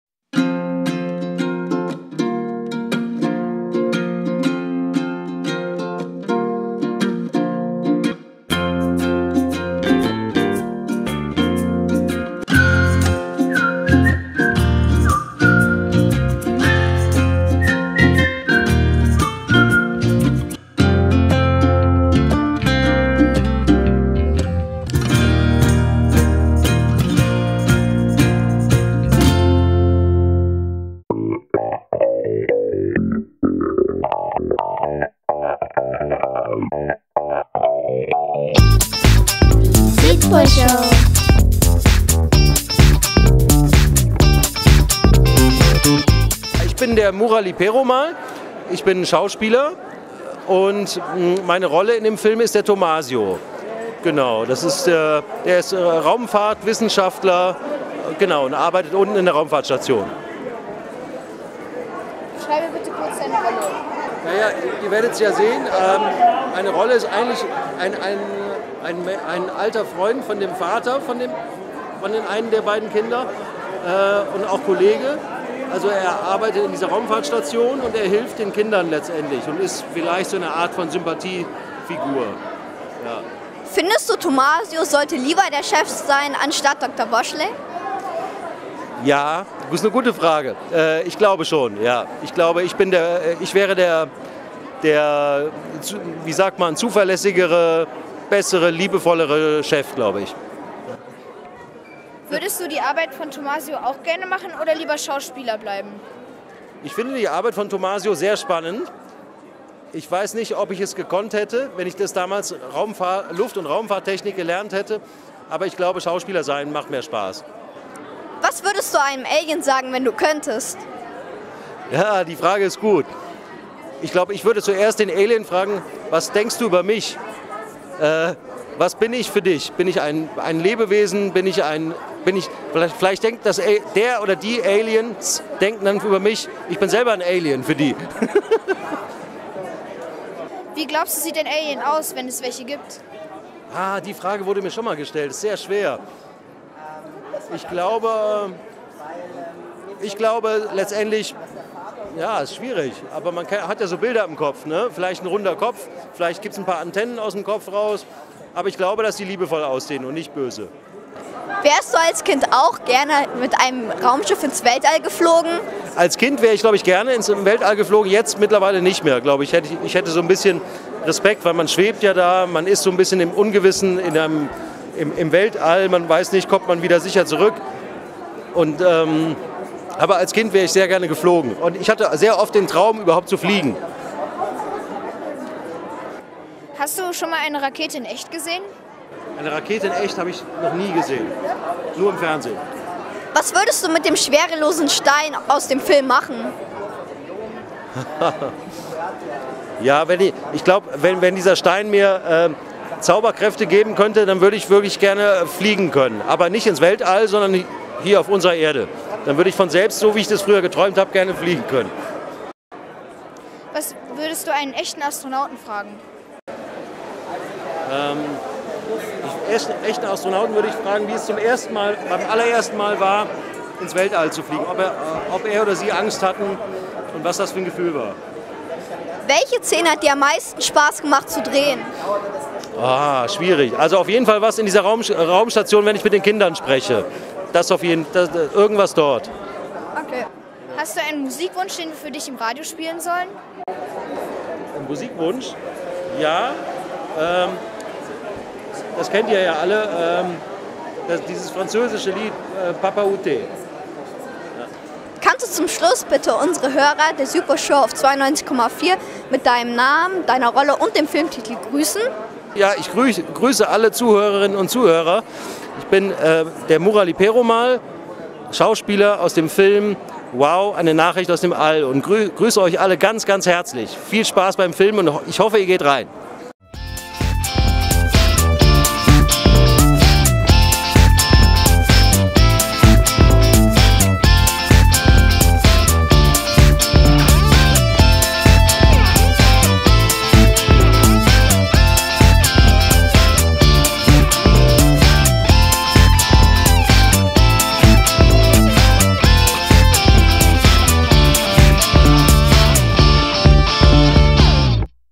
Interview mit Murali Perumal │Wow! Nachricht aus dem All│Südpolshow
Interview mit Murali Perumal auf der Premiere "Wow! Nachricht aus dem All". Er spielt Tomasio einen Mitarbeiter im Raumfahrtzenrum.
⛄ Oº°‘¨ Schaut doch mal hier vorbei!  ¨‘°ºO ⛄